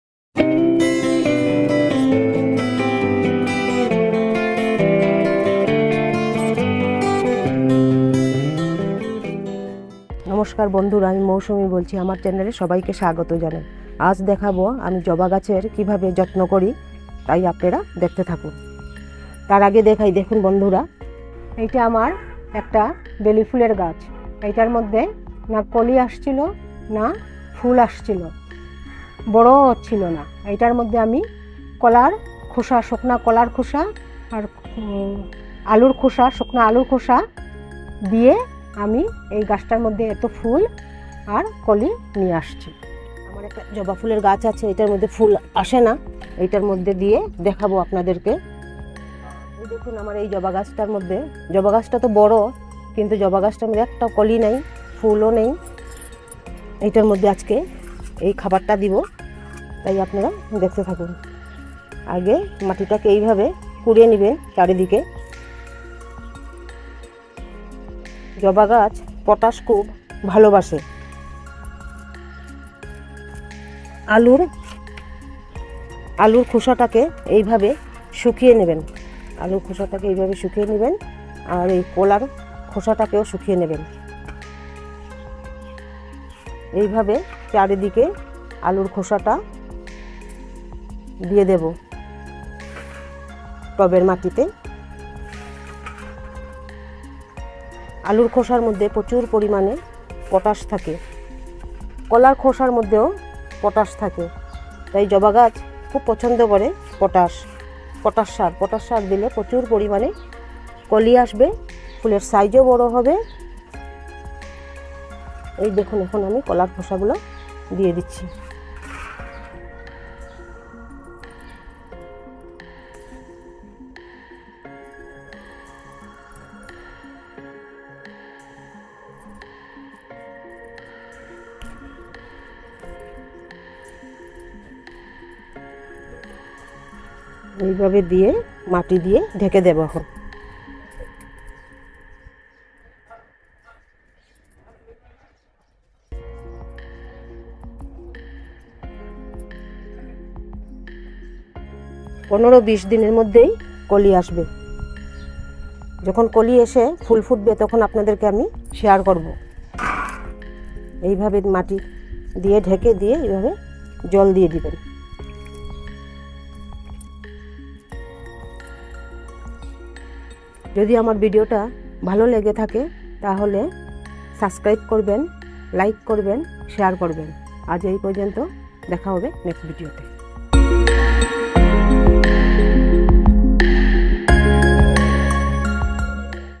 বাড়িতে জবা ফুলের যত্ন নেওয়ার সহজ উপায় | The Flora Cult
বাড়িতে জবা ফুলের যত্ন নেওয়ার সহজ উপায় | Easy way of caring of Hibiscus flower at home | The Flora Cult

ধন্যবাদ :)